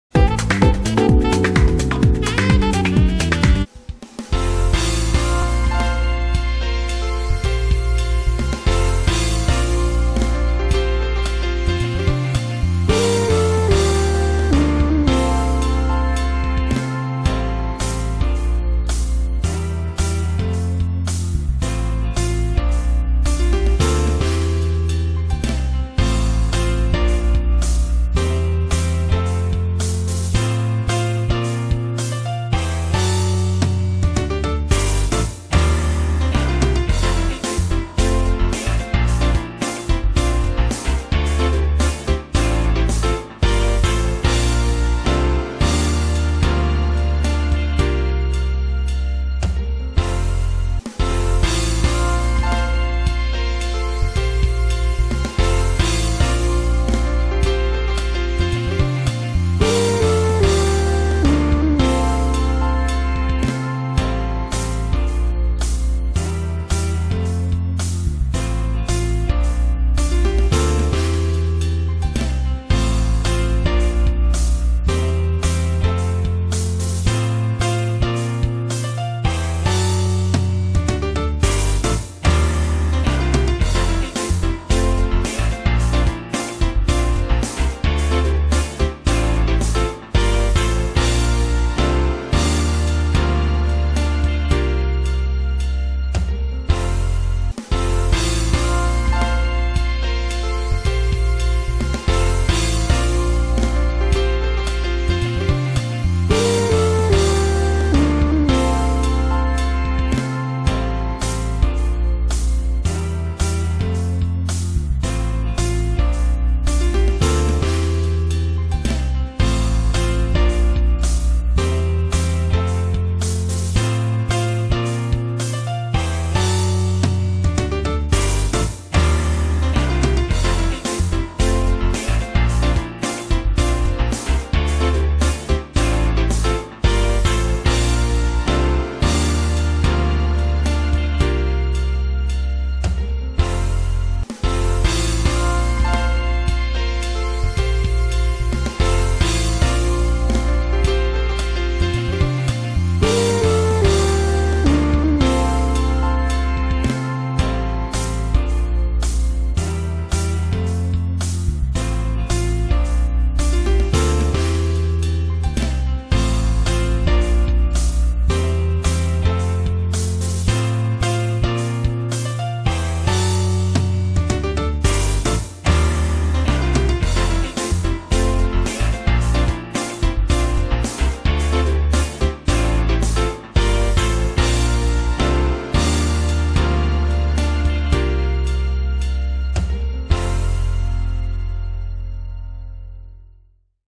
Adventures of guinea pig in the garden. Funny animals. Приключения морской свинки в саду
Продолжаются забеги нашего зверька в огороде.